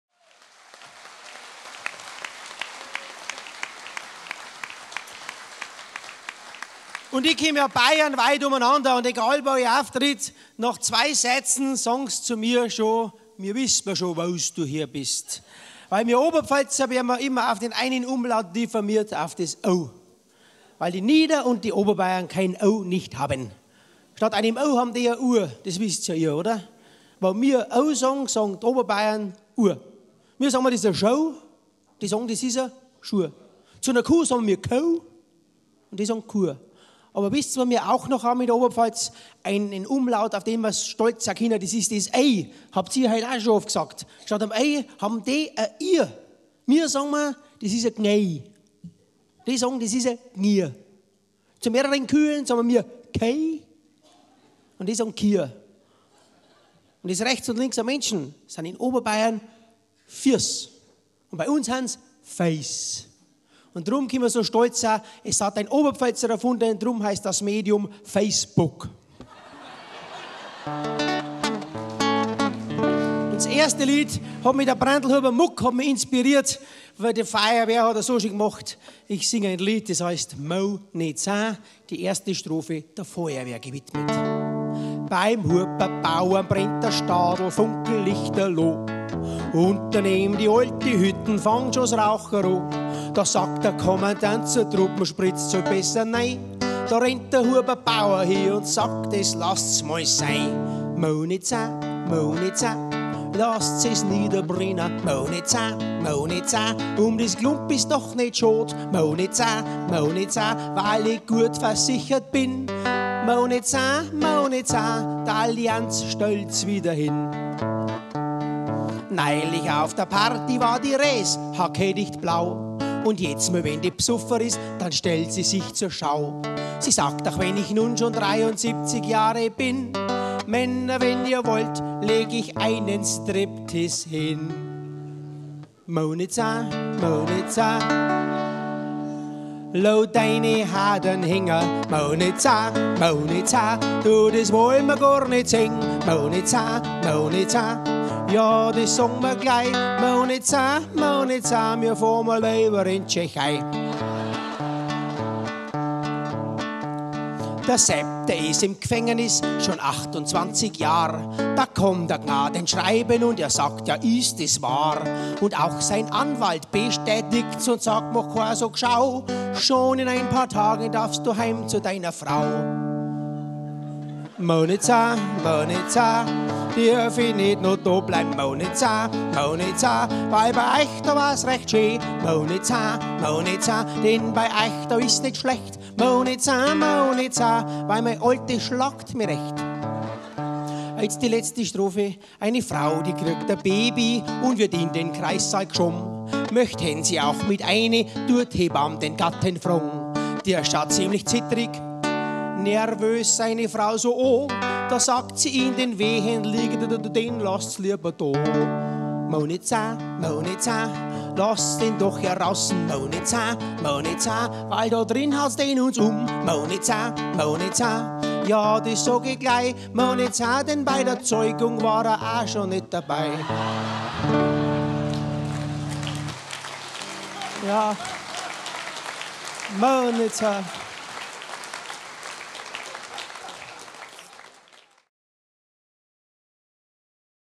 Josef "Bäff" Piendl über die Oberpfalz, Oberbayern und Niederbayern!
Humorist Bäff Piendl bei seinem Auftritt an der Oberpfälzer Witzemeisterschaft 2016 im Kolpinghaus in Regensburg.

Nähere Informationen rund um die Witzemeisterschaft auf unserer Facebookseite "Witzemeisterschaft" oder